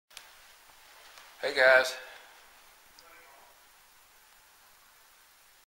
101 ParaCafe - 2FL Hey Guys Shut it Off
Kevin is filming on the 2nd floor and says "Hey guys" to the spirit. What follows is a male spirit saying "Shut it off". Assuming the spirit was referring the Kevin's camera.